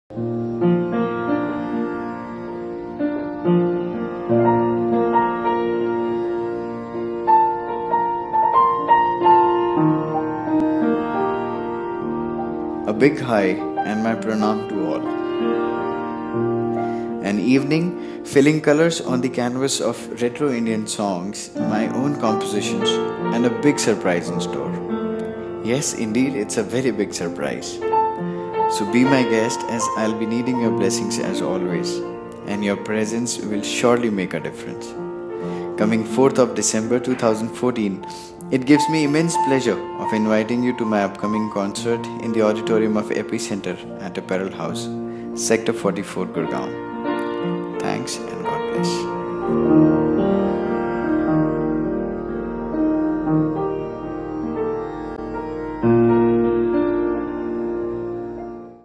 An ivitation by pianist Aman Bathla
"Pragaman" ODYSSEY OF A PIANIST
An Evening With PIANIST AMAN BATHLA
Experience and feel his spontaneous raga based compositions.
Bollywood songs
and a biiiiiig surprise.